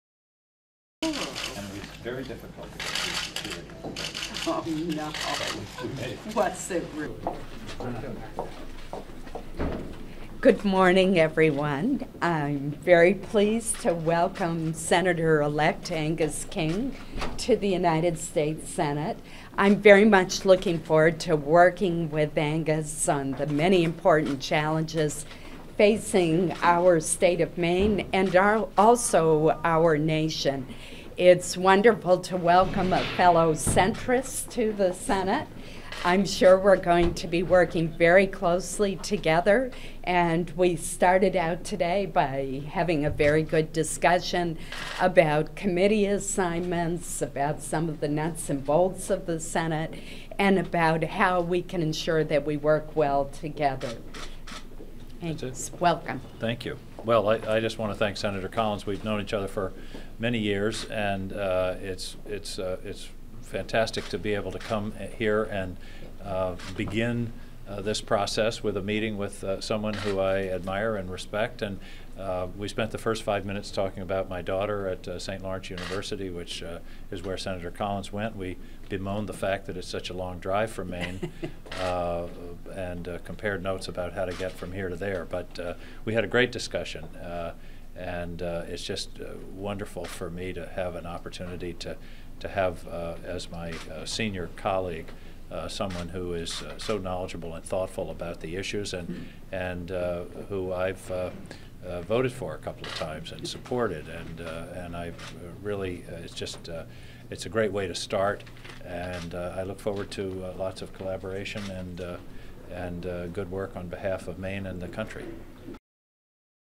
Senator Susan Collins welcomes Senator-Elect Angus King to Washington, DC
The two met in Senator Collins' Washignton, D.C. office on November 13, 2012 and discussed how they best can work together to represent Maine in the U.S. Senate.